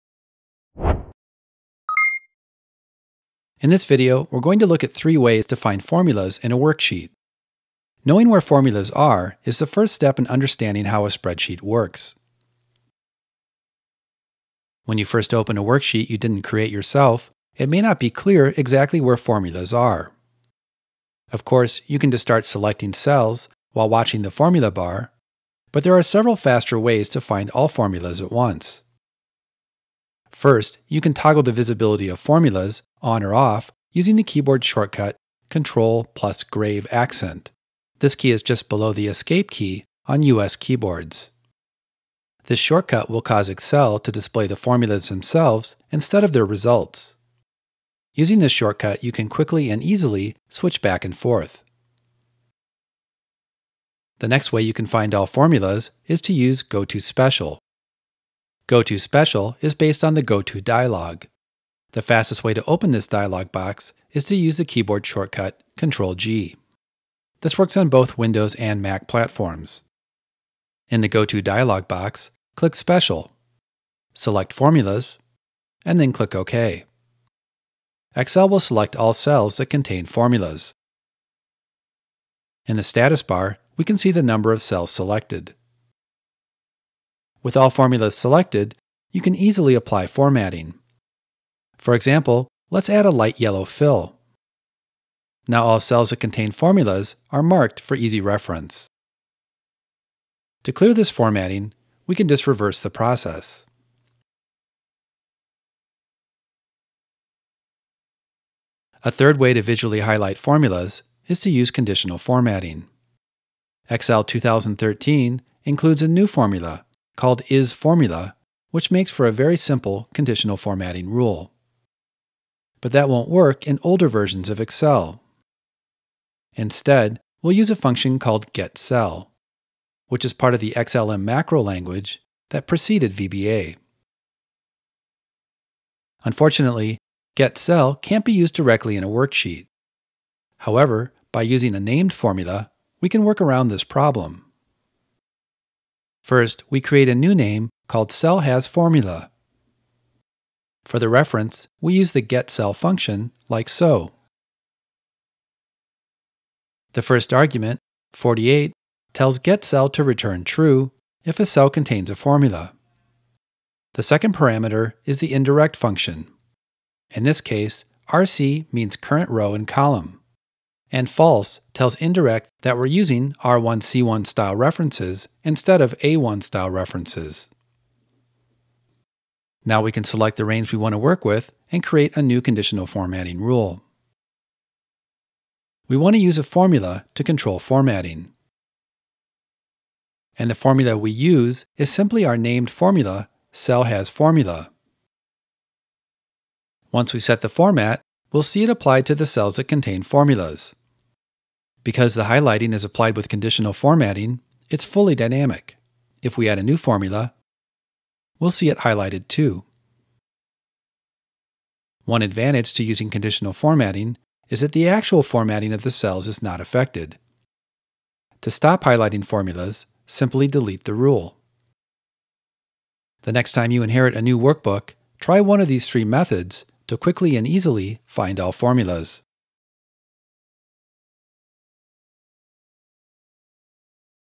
How to find and highlight formulas
In this video tip, we show you three ways to find formulas in a worksheet, including a tricky way to use conditional formatting and a named formula to highlight formulas.